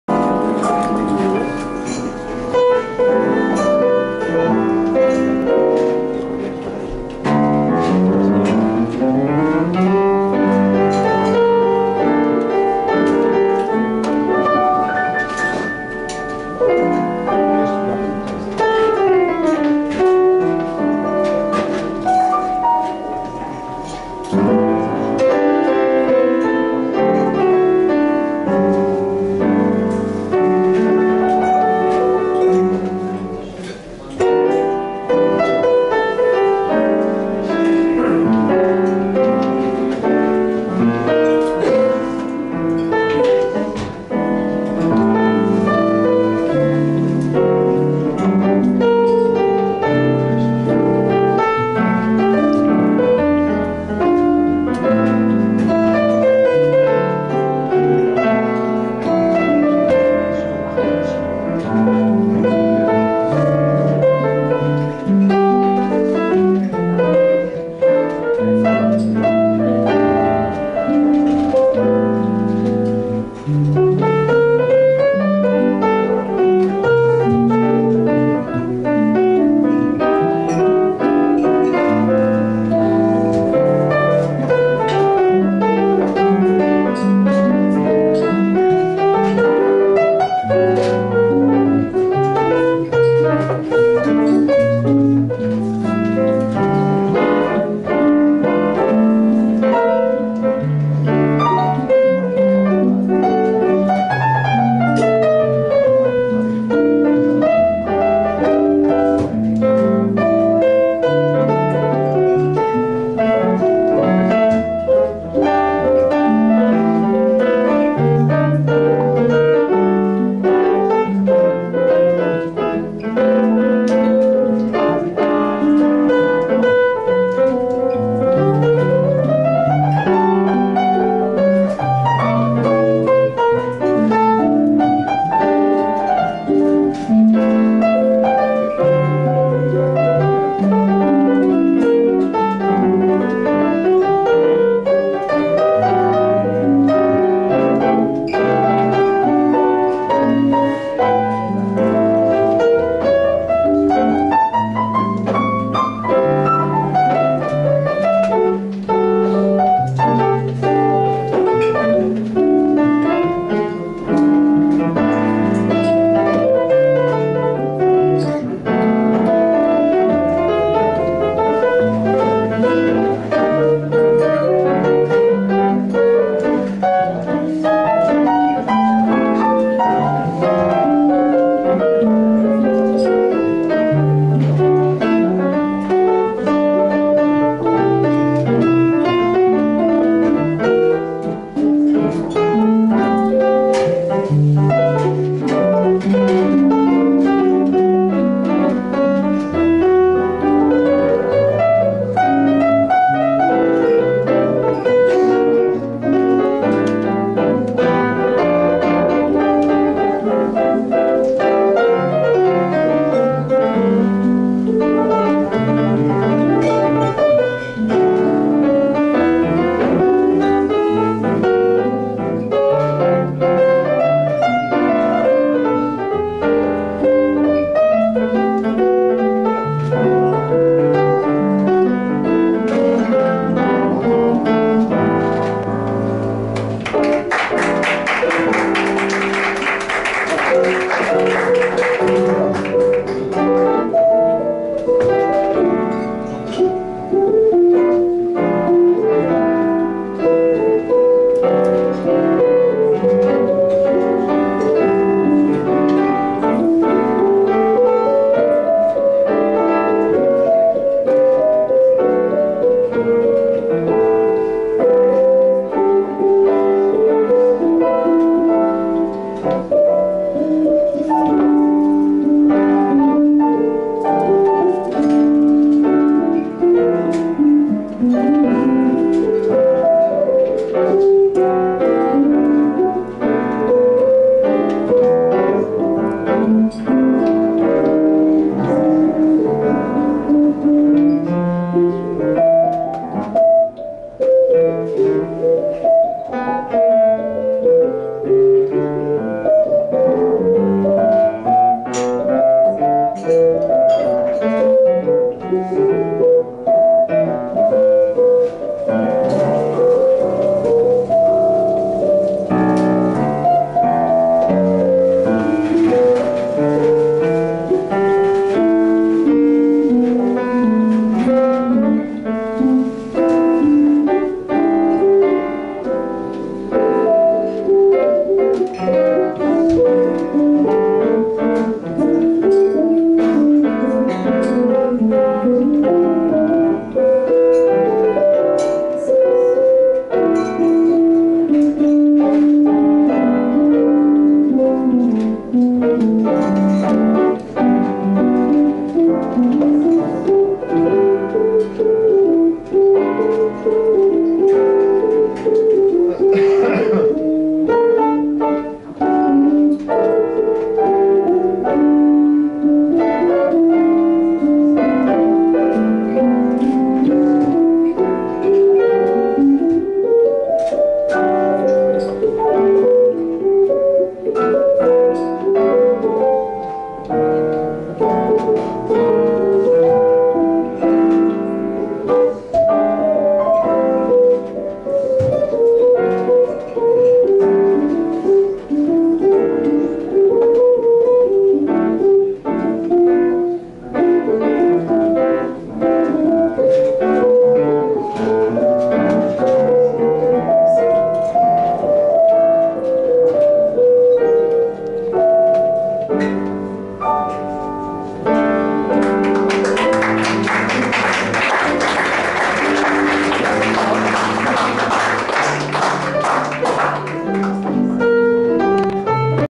Christian Molina Trio at Jimmy Glass Jazz Bar (1 of 2)
First theme played by Christian Molina on the venue at Jimmy Glass Jazz Bar (Valência, Spain) on the 3rd of october 2015.
A beautiful club with a great sprit and great music